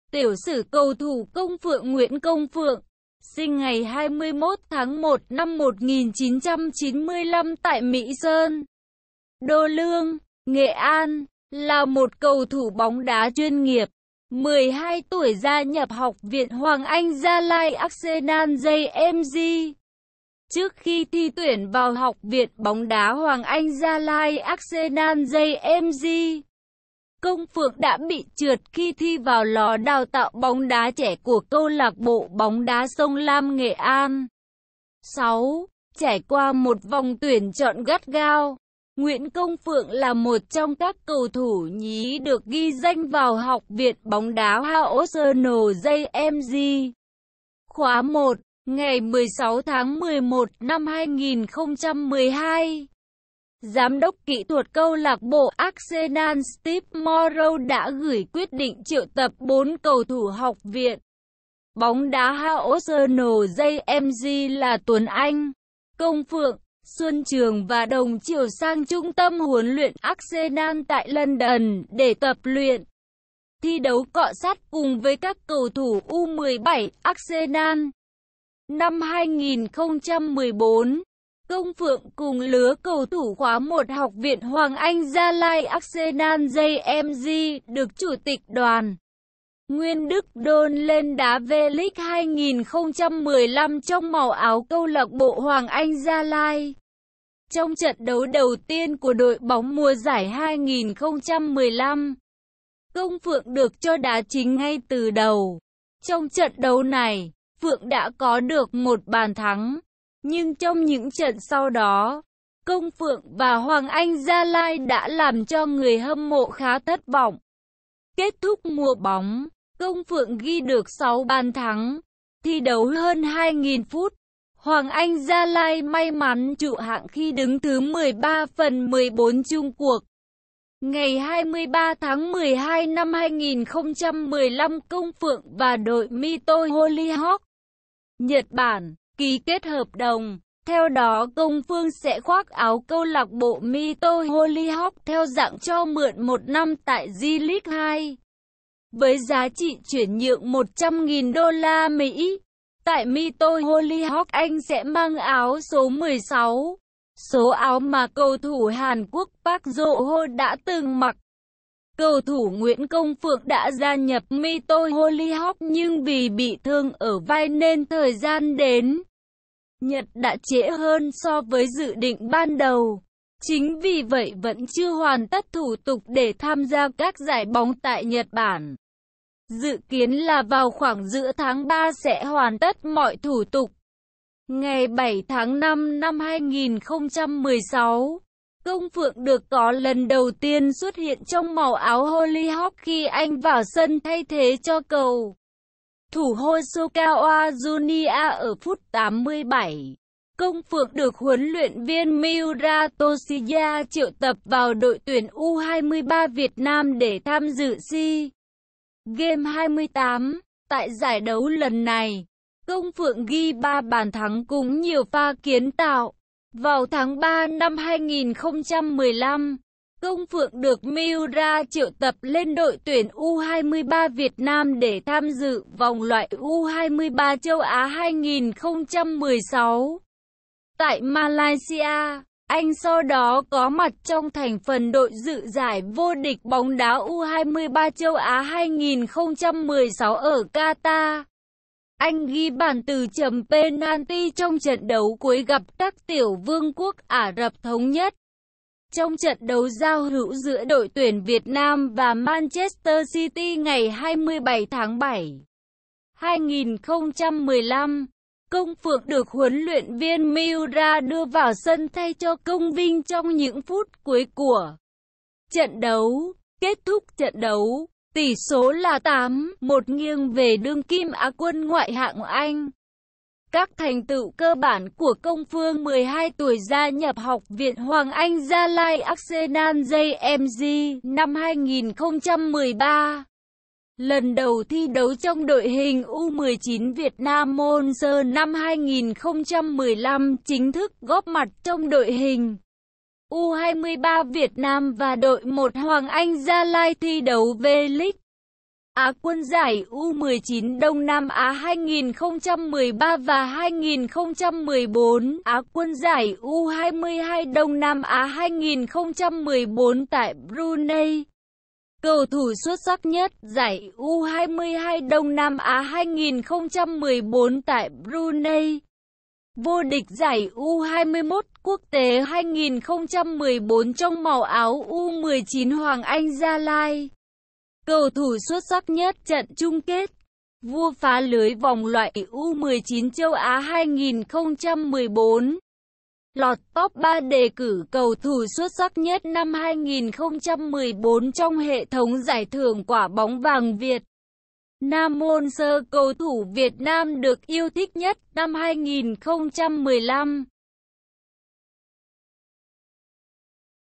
Tiểu sử Công Phượng | Thông tin về Công Phượng
Tiểu sử cầu thủ Công Phượng  Nguyễn Công Phượng (sinh ngày 21 tháng 1 năm 1995 tại Mỹ Sơn, Đô Lương, Nghệ An) là một cầu thủ bóng đá chuyên nghiệp.   12 tuổi gia nhập học viện HAGL Arsenal JMG: Trước khi thi tuyển vào học viện bóng đá Hoàng Anh Gia Lai - Arsenal JMG, Công Phượng đã bị trượt khi thi vào lò đào tạo bóng đá trẻ của câu lạc bộ bóng đá Sông Lam Nghệ An.[6] Trải qua 1 vòng tuyển chọn gắt gao, Nguyễn Công Phượng là một trong các cầu thủ nhí được ghi danh vào Học viện bóng đá HAGL-Arsenal JMG khóa 1.  Ngày 16 tháng 11 năm 2012, giám đốc kĩ thuật câu lạc bộ Arsenal Steve Morrow đã gửi quyết định triệu tập 4 cầu thủ Học viện bóng đá HAGL-Arsenal JMG là Tuấn Anh, Công Phượng, Xuân Trường và Đồng Triều sang Trung tâm huấn luyện Arsenal tại London để tập luyện, thi đấu cọ xát cùng với các cầu thủ U17 Arsenal.  Năm 2014, Công Phượng cùng lứa cầu thủ khóa một học viện Hoàng Anh Gia Lai – Arsenal – JMG được chủ tịch Đoàn Nguyên Đức đôn lên đá V-League 2015 trong màu áo câu lạc bộ Hoàng Anh Gia Lai.  Trong trận đấu đầu tiên của đội bóng mùa giải 2015, Công Phượng được cho đá chính ngay từ đầu. Trong trận đấu này, Phượng đã có được 1 bàn thắng. Nhưng trong những trận sau đó, Công Phượng và Hoàng Anh Gia Lai đã làm cho người hâm mộ khá thất vọng.  Kết thúc mùa bóng, Công Phượng ghi được 6 bàn thắng, thi đấu hơn 2000 phút. Hoàng Anh Gia Lai may mắn trụ hạng khi đứng thứ 13/14 chung cuộc.  Ngày 23 tháng 12 năm 2015 Công Phượng và đội Mito Hollyhock (Nhật Bản) ký kết hợp đồng, theo đó Công Phương sẽ khoác áo CLB Mito Hollyhock theo dạng cho mượn một năm tại J-League 2, với giá trị chuyển nhượng 100.000 USD. Tại Mito Hollyhock anh sẽ mang áo số 16, số áo mà cầu thủ Hàn Quốc Park Joo-Ho đã từng mặc.  Cầu thủ Nguyễn Công Phượng đã gia nhập Mito Hollyhock nhưng vì bị thương ở vai nên thời gian đến Nhật đã trễ hơn so với dự định ban đầu, chính vì vậy vẫn chưa hoàn tất thủ tục để tham gia các giải bóng tại Nhật Bản. Dự kiến là vào khoảng giữa tháng 3 sẽ hoàn tất mọi thủ tục.  Ngày 7 tháng 5 năm 2016, Công Phượng được có lần đầu tiên xuất hiện trong màu áo Hollyhock khi anh vào sân thay thế cho cầu thủ Hosokawa Junya ở phút 87.  Công Phượng được huấn luyện viên Miura Toshiya triệu tập vào đội tuyển U23 Việt Nam để tham dự SEA Games 28. Tại giải đấu lần này, Công Phượng ghi 3 bàn thắng cùng nhiều pha kiến tạo.  Vào tháng 3 năm 2015, Công Phượng được Miura triệu tập lên đội tuyển U-23 Việt Nam để tham dự vòng loại U-23 châu Á 2016 tại Malaysia. Anh sau đó có mặt trong thành phần đội dự giải vô địch bóng đá U-23 châu Á 2016 ở Qatar. Anh ghi bàn từ chấm penalty trong trận đấu cuối gặp Các Tiểu vương quốc Ả Rập Thống nhất.  Trong trận đấu giao hữu giữa đội tuyển Việt Nam và Manchester City ngày 27 tháng 7, 2015, Công Phượng được huấn luyện viên Miura đưa vào sân thay cho Công Vinh trong những phút cuối của trận đấu. Kết thúc trận đấu, tỉ số là 8-1 nghiêng về đương kim á quân Ngoại hạng Anh.  Các thành tựu cơ bản của Công Phương  12 tuổi gia nhập học viện HAGL Arsenal JMG.    Năm 2013 lần đầu thi đấu trong đội hình U19 Việt Nam.    Năm 2015 chính thức góp mặt trong đội hình U23 Việt Nam & đội một HAGL thi đấu V-League.   Á quân giải U19 Đông Nam Á 2013 & 2014.    Á quân giải U22 Đông Nam Á 2014 tại Brunei.   Cầu thủ xuất sắc nhất giải U22 Đông Nam Á 2014 tại Brunei.   Vô địch giải U21 Quốc tế 2014 trong màu áo U19 HAGL (Cầu thủ xuất sắc nhất trận chung kết)   Vua phá lưới vòng loại U19 châu Á 2014.   Lọt top 3 đề cử Cầu thủ xuất sắc nhất năm 2014 trong hệ thống giải thưởng Quả bóng vàng Việt Nam.   Cầu thủ Việt Nam được yêu thích nhất năm 2015